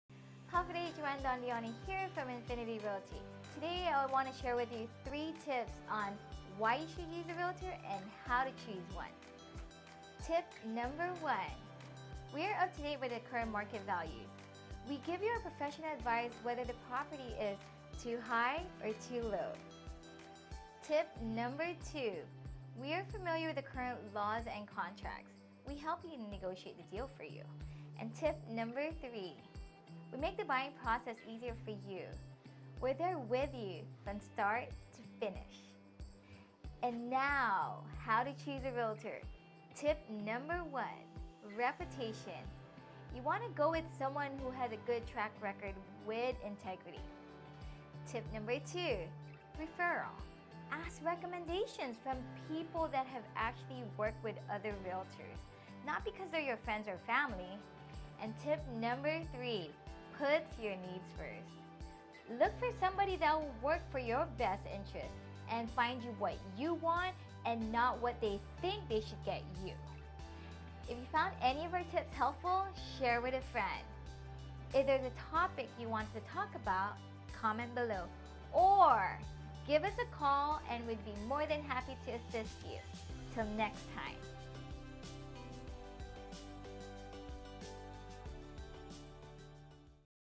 Choosing a REALTOR - INFINITY REALTY GUAM
Here is our TOP 3 tips on why you need a REALTOR® and how to choose one.